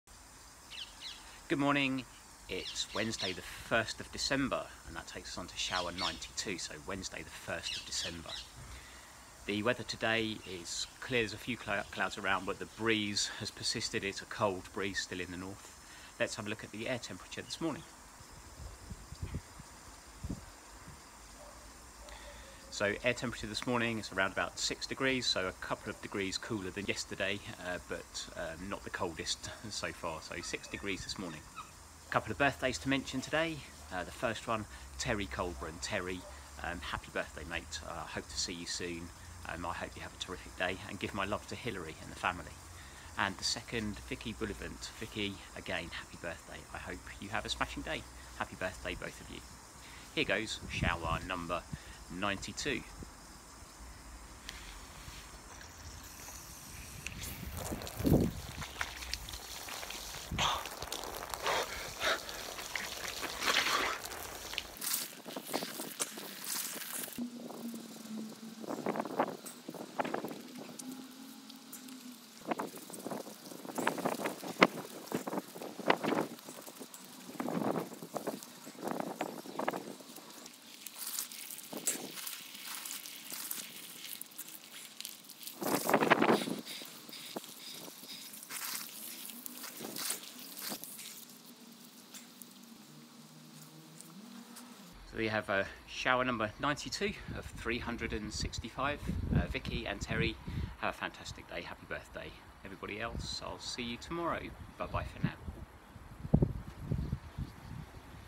Outdoor Shower Challenge - Day 92/365
𝗗𝗮𝘆 𝟵𝟮/𝟯𝟲𝟱 – 𝗢𝘂𝘁𝗱𝗼𝗼𝗿 𝗖𝗼𝗹𝗱-𝗪𝗮𝘁𝗲𝗿 𝗦𝗵𝗼𝘄𝗲𝗿𝘀 𝗳𝗼𝗿 𝗮 𝗬𝗲𝗮𝗿

. . . 𝗮𝗻𝗱 𝘄𝗲'𝗿𝗲 𝗶𝗻𝘁𝗼 𝗗𝗲𝗰𝗲𝗺𝗯𝗲𝗿! 𝗪𝗵𝘆 𝗻𝗼𝘁 𝗺𝗮𝗸𝗲 𝗺𝗲 𝘆𝗼𝘂𝗿 𝗮𝗱𝘃𝗲𝗻𝘁 𝗰𝗮𝗹𝗲𝗻𝗱𝗮𝗿 𝘁𝗵𝗶𝘀 𝘆𝗲𝗮𝗿?!

£𝟮𝟰𝟲𝟱.𝟴𝟰 (𝗶𝗻𝗰. 𝗚𝗶𝗳𝘁 𝗔𝗶𝗱) 𝗼𝗳 £𝟭𝟬𝟬𝟬𝟬 𝗧𝗮𝗿𝗴𝗲𝘁; 𝗿𝗮𝗶𝘀𝗲𝗱 𝘀𝗼 𝗳𝗮𝗿! 𝗧𝗵𝗮𝗻𝗸 𝘆𝗼𝘂 𝘀𝗼 𝘀𝗼 𝗺𝘂𝗰𝗵; 𝗜'𝗺 𝘁𝗵𝗿𝗶𝗹𝗹𝗲𝗱 𝘄𝗶𝘁𝗵 𝘁𝗵𝗲 𝗳𝘂𝗻𝗱𝗿𝗮𝗶𝘀𝗶𝗻𝗴 𝗽𝗿𝗼𝗴𝗿𝗲𝘀𝘀!

• 𝗥𝗮𝗶𝘀𝗶𝗻𝗴 𝗠𝗼𝗻𝗲𝘆 𝗳𝗼𝗿 𝘁𝗵𝗲 𝗥𝗡𝗟𝗜
• 𝗥𝗮𝗶𝘀𝗶𝗻𝗴 𝗔𝘄𝗮𝗿𝗲𝗻𝗲𝘀𝘀 𝗳𝗼𝗿 𝗗𝗿𝗼𝘄𝗻𝗶𝗻𝗴 𝗣𝗿𝗲𝘃𝗲𝗻𝘁𝗶𝗼𝗻
• 𝗘𝗻𝗰𝗼𝘂𝗿𝗮𝗴𝗶𝗻𝗴 𝗿𝗲𝘀𝗽𝗼𝗻𝘀𝗶𝗯𝗹𝗲 𝘂𝘀𝗲 𝗼𝗳 𝘁𝗵𝗲 𝗱𝗿𝗮𝗶𝗻𝗮𝗴𝗲 𝗮𝗻𝗱 𝘀𝗲𝘄𝗲𝗿 𝘀𝘆𝘀𝘁𝗲𝗺𝘀

𝗗𝗮𝘆 𝟵𝟮 - 𝗜'𝘃𝗲 𝗺𝗮𝗱𝗲 𝗶𝘁 𝗶𝗻𝘁𝗼 𝗗𝗲𝗰𝗲𝗺𝗯𝗲𝗿 𝗮𝗻𝗱 𝘁𝗵𝗲 𝗳𝗶𝗿𝘀𝘁 𝗱𝗮𝘆 𝗼𝗳 𝘁𝗵𝗲 𝗺𝗼𝗻𝘁𝗵 𝗵𝗮𝘀 𝗯𝗲𝗲𝗻 𝗿𝗲𝗹𝗮𝘁𝗶𝘃𝗲𝗹𝘆 𝗸𝗶𝗻𝗱 𝘁𝗼 𝗺𝗲. 𝗜𝘁'𝘀 𝟲 𝗱𝗲𝗴𝗿𝗲𝗲𝘀 𝗮𝗻𝗱, 𝘄𝗵𝗶𝗹𝘀𝘁 𝘁𝗵𝗲𝗿𝗲'𝘀 𝗮 𝗯𝗿𝗲𝗲𝘇𝗲, 𝗜 𝗵𝗮𝘃𝗲 𝗵𝗮𝗱 𝗳𝗮𝗿 𝘄𝗼𝗿𝘀𝗲.

𝗧𝘄𝗼 𝗯𝗶𝗿𝘁𝗵𝗱𝗮𝘆𝘀 𝗴𝗲𝘁 𝗮 𝗺𝗲𝗻𝘁𝗶𝗼𝗻 𝗶𝗻 𝘁𝗵𝗲 𝘃𝗶𝗱𝗲𝗼 𝘁𝗼𝗱𝗮𝘆: 𝗧𝗲𝗿𝗿𝘆 𝗖𝗼𝗹𝗯𝗿𝗮𝗻; 𝗛𝗮𝗽𝗽𝘆 𝗕𝗶𝗿𝘁𝗵𝗱𝗮𝘆 𝗧𝗲𝗿𝗿𝘆! 𝗜 𝗵𝗼𝗽𝗲 𝘆𝗼𝘂'𝘃𝗲 𝗯𝗲𝗲𝗻 𝗵𝗮𝘃𝗶𝗻𝗴 𝗮𝗻𝗱 𝗮𝗺𝗮𝘇𝗶𝗻𝗴 𝗱𝗮𝘆, 𝗜 𝗵𝗼𝗽𝗲 𝘁𝗼 𝘀𝗲𝗲 𝘆𝗼𝘂 𝘀𝗼𝗼𝗻. 𝗔𝗻𝗱 𝗩𝗶𝗸𝗸𝗶 𝗕𝘂𝗹𝗹𝗶𝘃𝗮𝗻𝘁; 𝗛𝗮𝗽𝗽𝘆 𝗕𝗶𝗿𝘁𝗵𝗱𝗮𝘆 𝗩𝗶𝗸𝗸𝗶! 𝗜'𝘃𝗲 𝗻𝗼𝘁 𝘀𝗲𝗲𝗻 𝘆𝗼𝘂 𝗳𝗼𝗿 𝗮 𝗹𝗼𝗻𝗴 𝘁𝗶𝗺𝗲 𝗯𝘂𝘁 𝗜 𝗵𝗼𝗽𝗲 𝘆𝗼𝘂'𝗿𝗲 𝘄𝗲𝗹𝗹 𝗮𝗻𝗱 𝘁𝗵𝗮𝘁 𝘆𝗼𝘂'𝘃𝗲 𝗵𝗮𝗱 𝗮 𝗴𝗿𝗲𝗮𝘁 𝗱𝗮𝘆 (𝗮𝗽𝗼𝗹𝗼𝗴𝗶𝗲𝘀 𝗧𝗲𝗿𝗿𝘆 𝗮𝗻𝗱 𝗩𝗶𝗸𝗸𝗶; 𝘁𝗵𝗶𝘀 𝗶𝘀 𝗽𝗼𝘀𝘁𝗲𝗱 𝗮 𝗱𝗮𝘆 𝗹𝗮𝘁𝗲 𝗮𝘀 𝗜 𝗵𝗮𝗱𝗻'𝘁 𝗵𝗮𝗱 𝗮𝗻 𝗼𝗽𝗽𝗼𝗿𝘁𝘂𝗻𝗶𝘁𝘆 𝘁𝗼 𝗲𝗱𝗶𝘁 𝗲𝘁𝗰. 𝘂𝗻𝘁𝗶𝗹 𝗻𝗼𝘄).

𝗔𝘀 𝗲𝘃𝗲𝗿, 𝘁𝗵𝗮𝗻𝗸 𝘆𝗼𝘂 𝘀𝗼 𝗺𝘂𝗰𝗵 𝗳𝗼𝗿 𝘆𝗼𝘂𝗿 𝗼𝗻𝗴𝗼𝗶𝗻𝗴 𝘀𝘂𝗽𝗽𝗼𝗿𝘁!